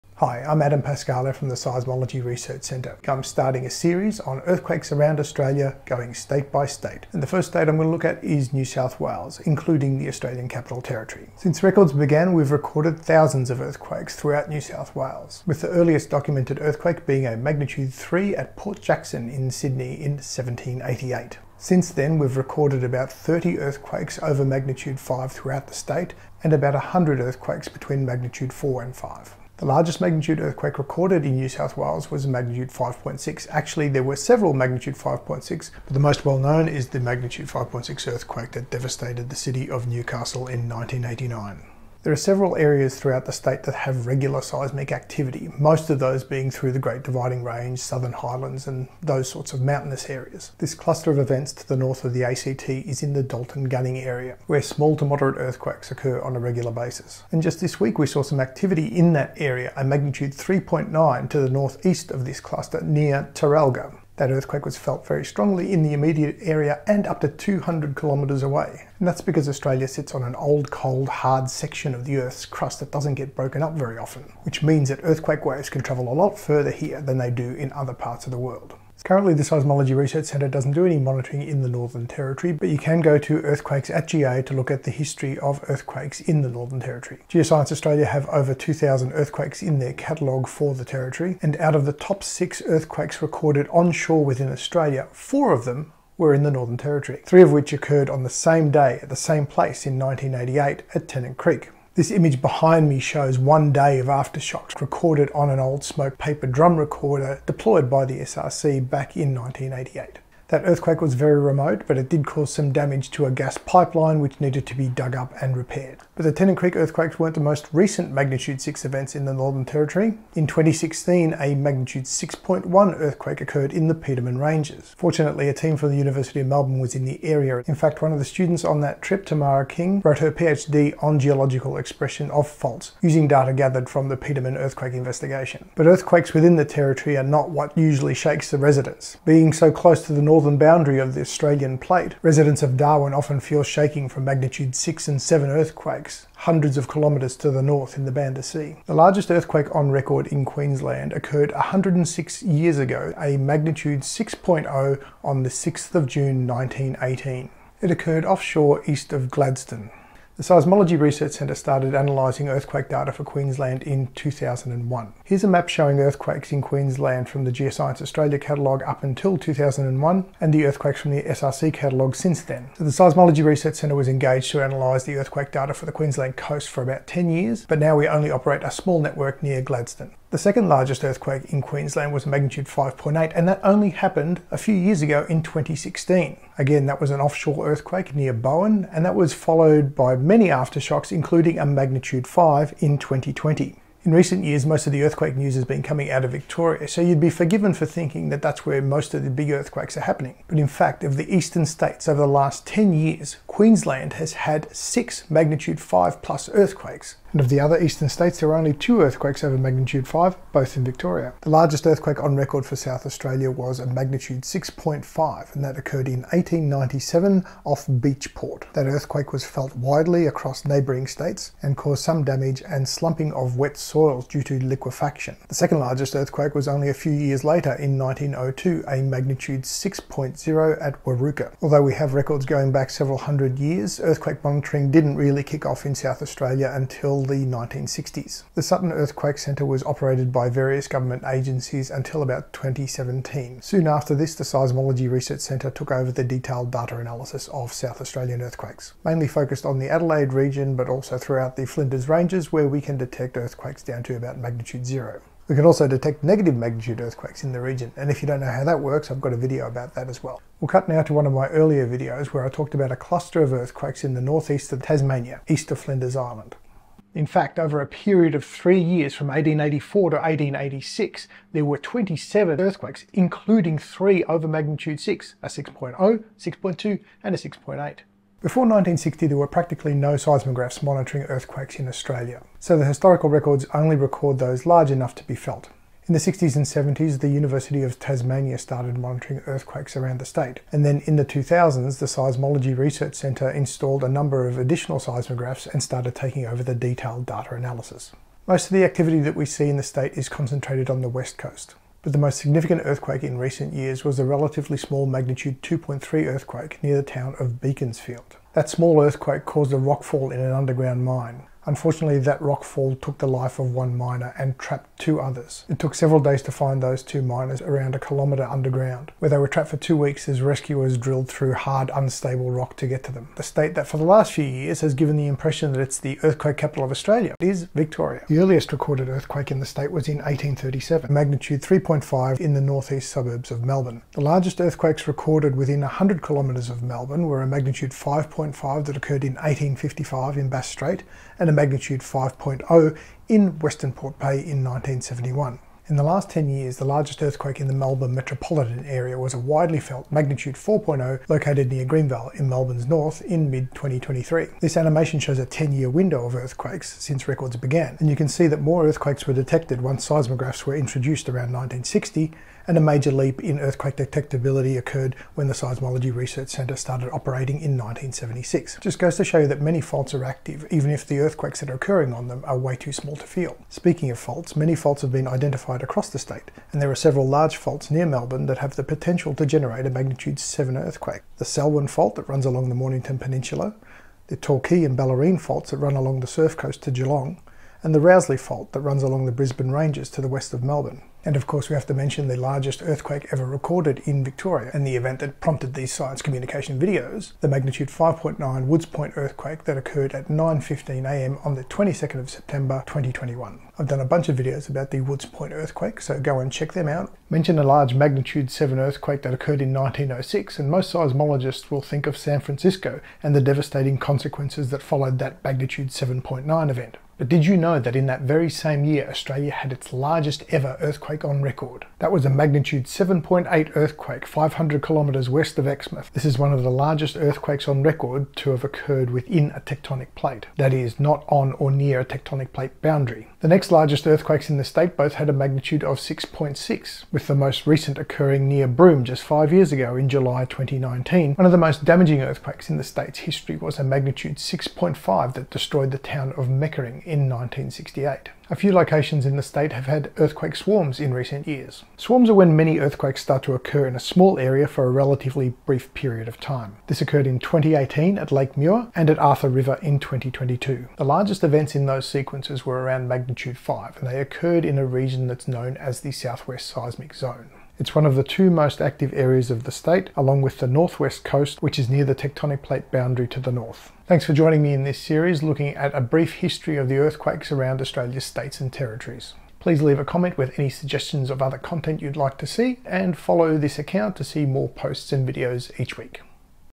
A Brief History of Earthquakes Around Australia
We’ve combined our recent weekly video series on each Australian state’s earthquakes into a brief history of significant seismic events around the country. States appear in alphabetical order, so use the chapter markers to find what you’re looking for.

0:00 New South Wales & the A.C.T.
1:18 Northern Territory
2:28 Queensland
3:31 South Australia
4:20 Tasmania
5:23 Victoria
6:47 Western Australia
7:53 Comments & suggestions